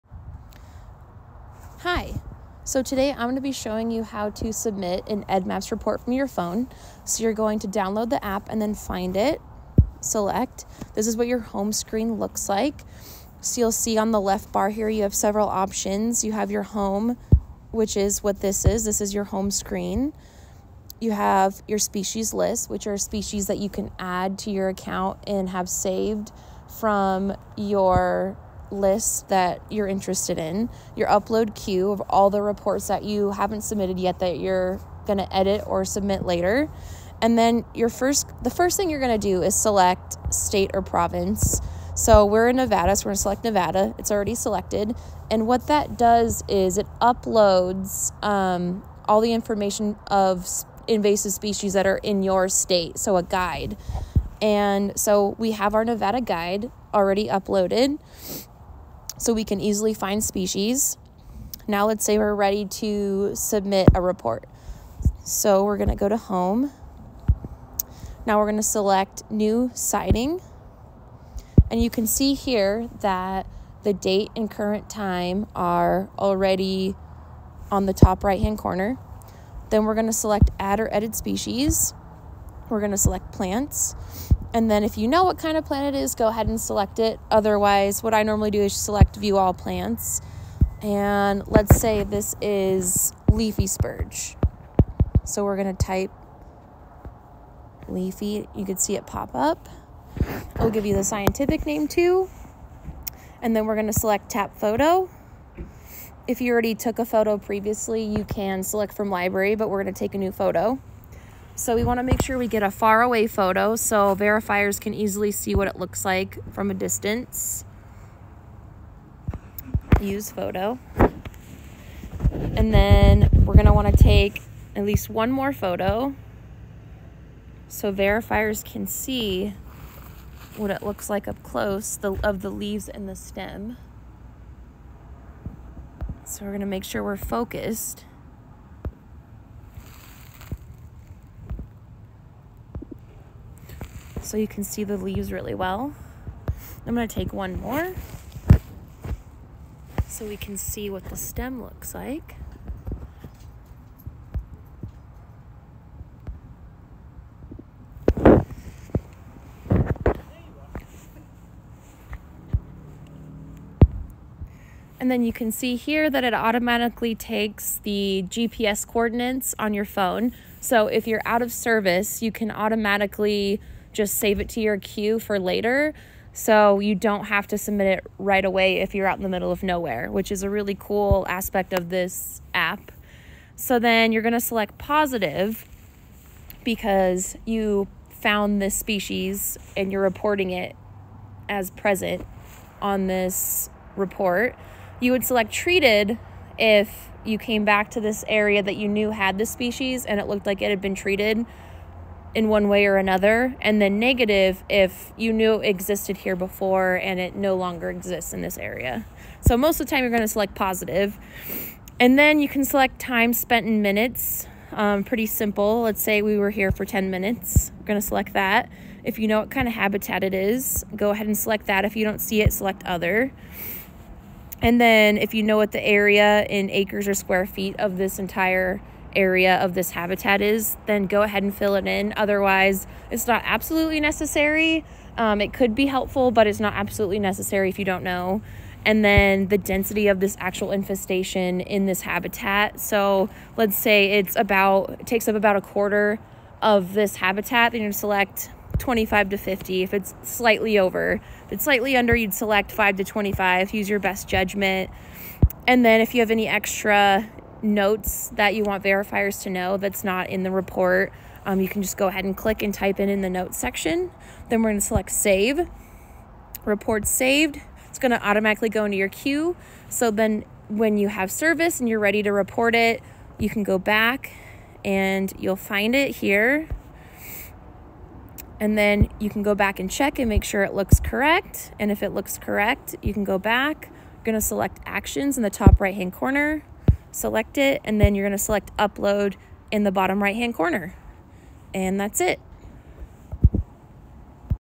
How to use EDDMapS
Follow this tutorial to learn how to report noxious weeds.
A Nevada Department of Agriculture video.